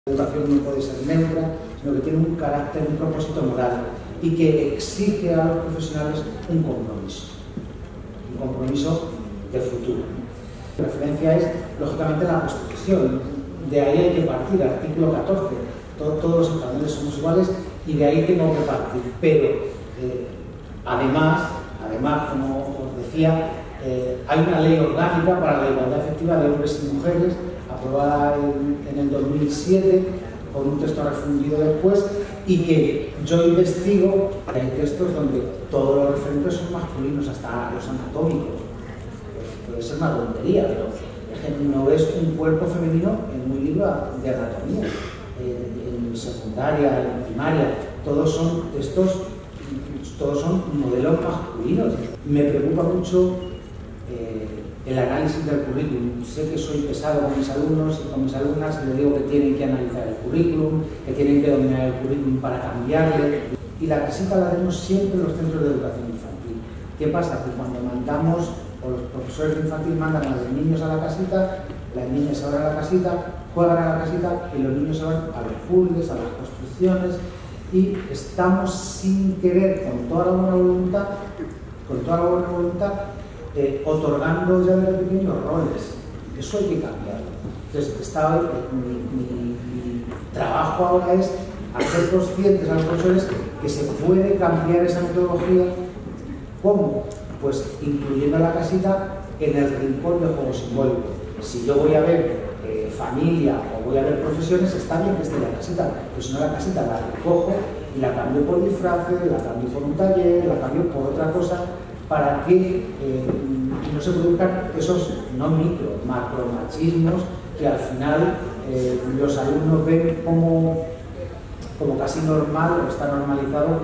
Javier Rodríguez Torres en las II Jornadas "Soy Mujer" de encastillalamancha.es
Javier Rodríguez Torres, profesor de Didáctica y Organización Escolar del Departamento de Pedagogía de la UCLM, en las II Jornadas "Soy Mujer" de encastillalamancha.es.